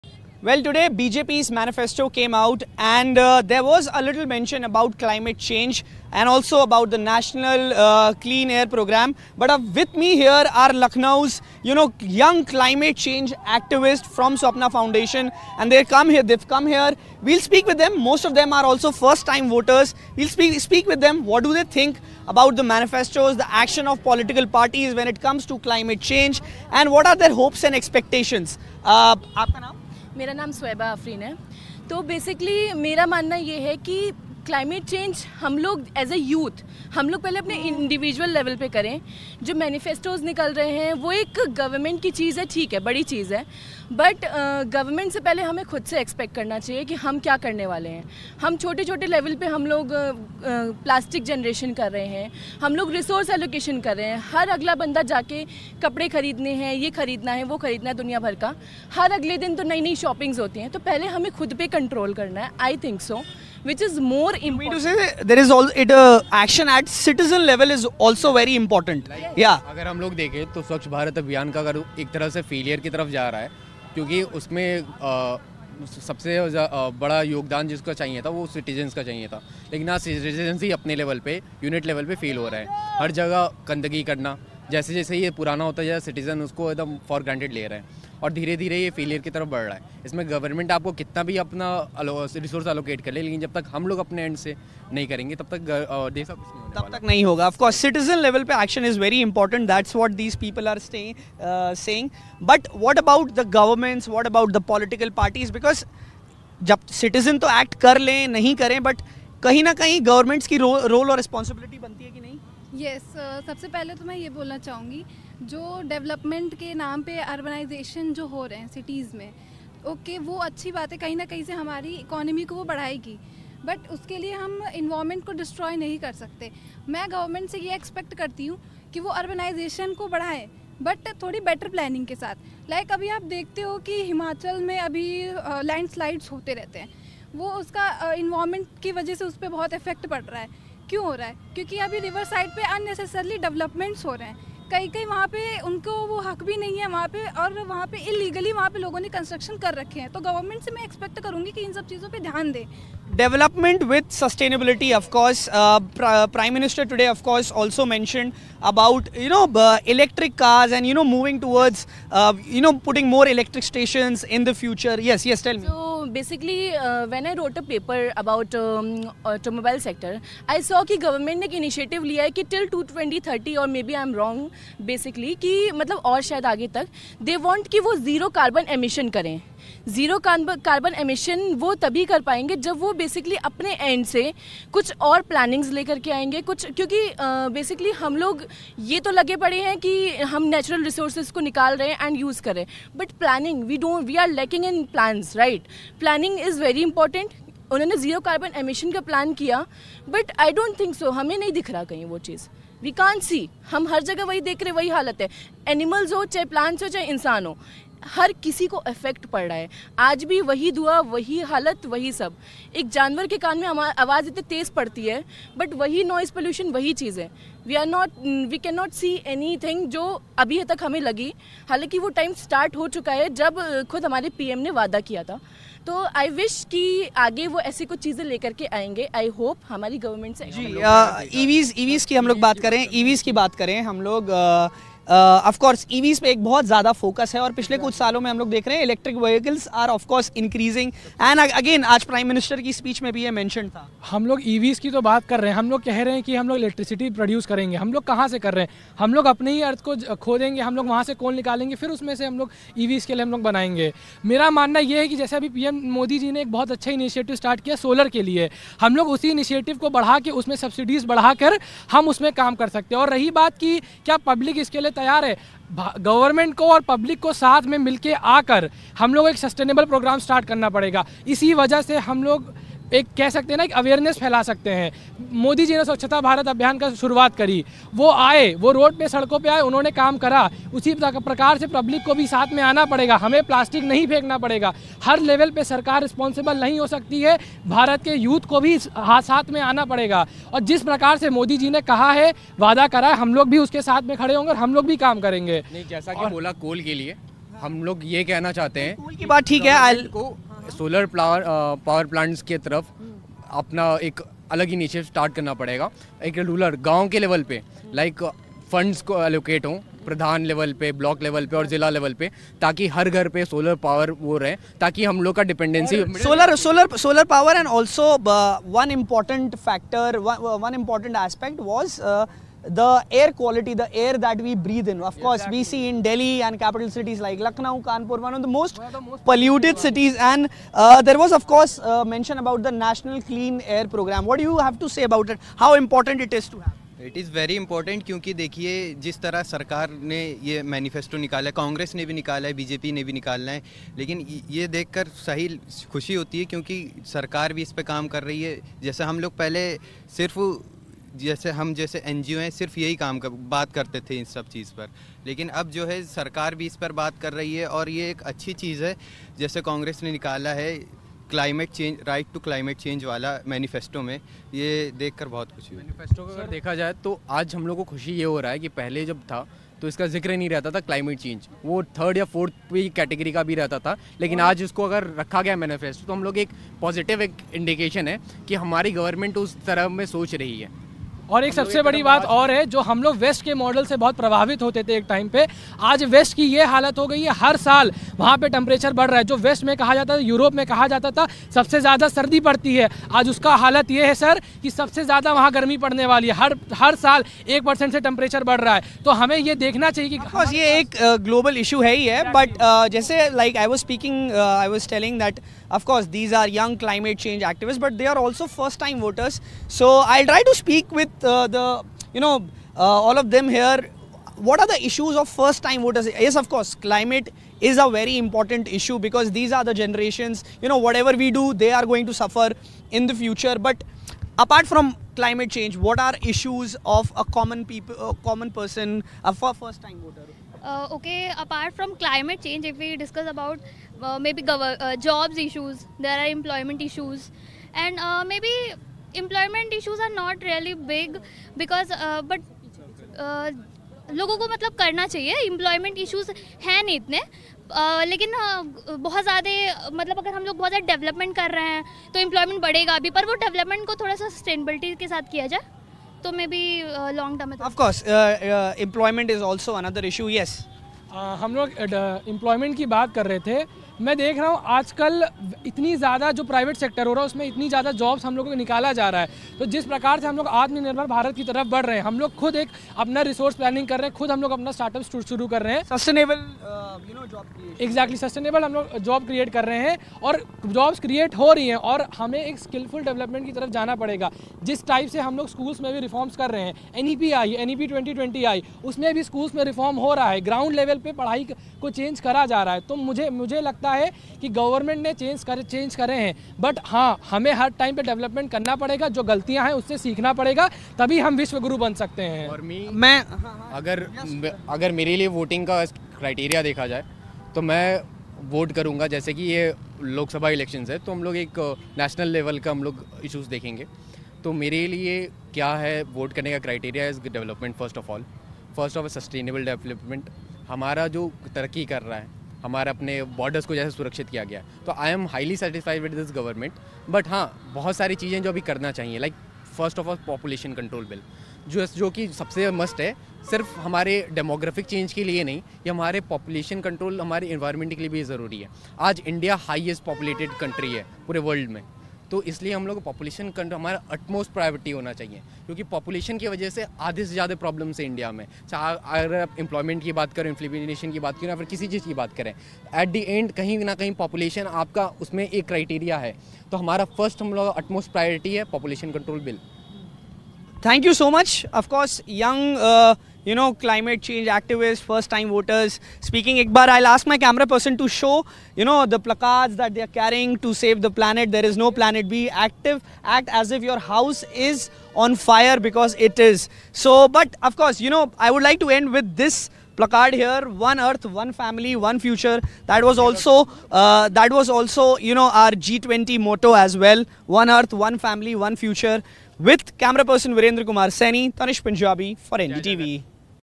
Lok Sabha Elections 2024 | How Serious Political Parties Are About Climate Change
Ahead of Lok Sabha polls, first-time voters in Lucknow speak about climate change in manifestos released by the BJP and other political parties. Let's listen in.